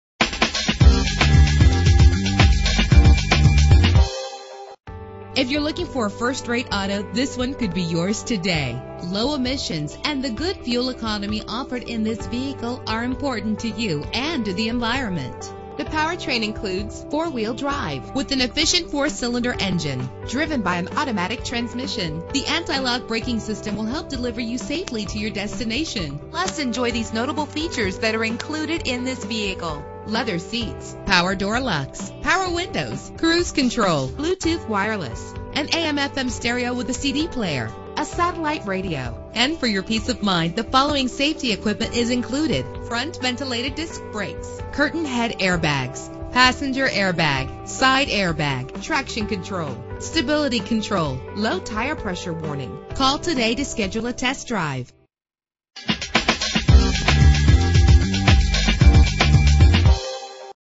2014 Mitsubishi Outlander Sport - Totowa NJ
Year: 2014
Make: Mitsubishi
Model: Outlander Sport
Trim: SE
Engine: 2.0 liter inline 4 cylinder 16 valve
Transmission: Automatic CVT
Color: Silver
Mileage: 10
Address:
440 Route 46 East